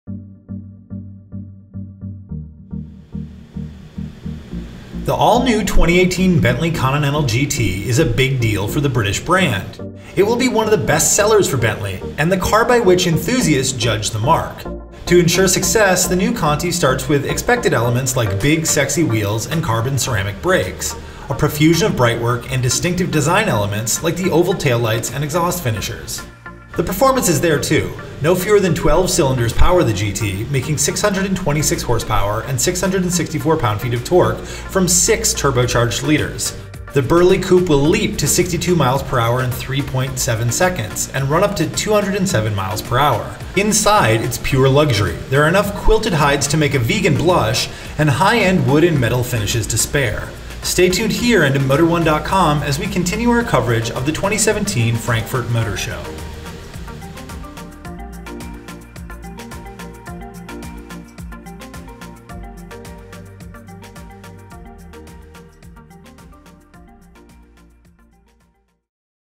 2018 Bentley Continental GT: 2017 Frankfurt Motor Show
We take you through all the essential details on the 2018 Bentley Continental GT from the 2017 Frankfurt Motor Show.

For more on the 2018 Bentley Continental GT read the full article